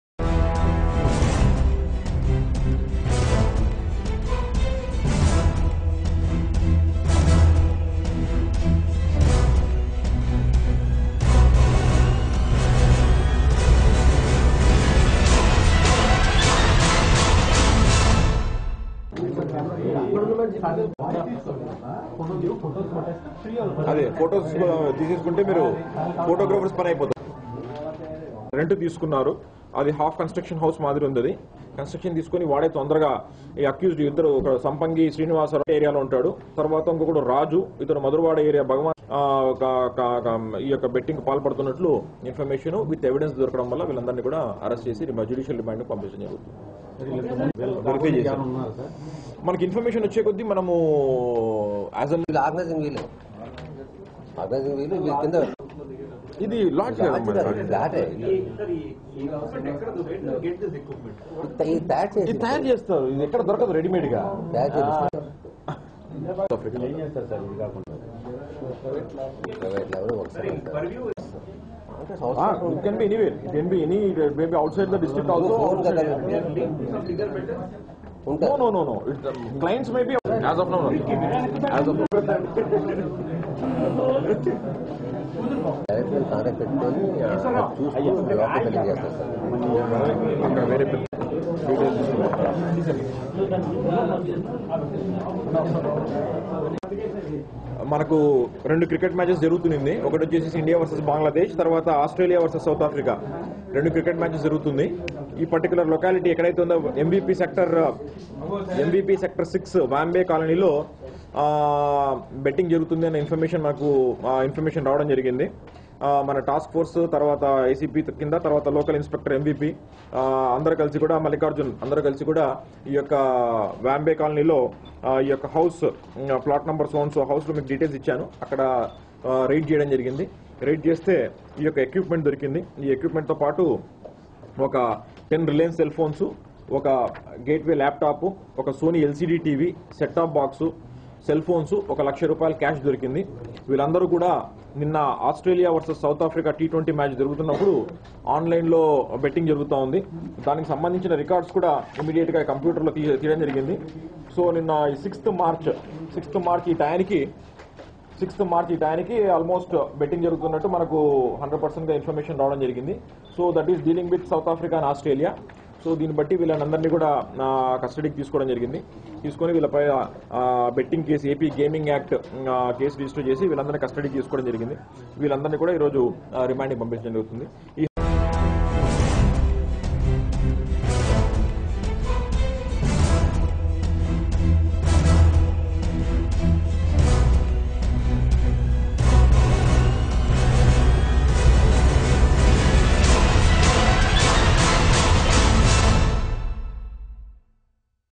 crime
క్రికెట్ బెట్టింగ్‌లో దోరికిన ప్రభుదులు కటకటల వెనకి పంపిన పోలిసులు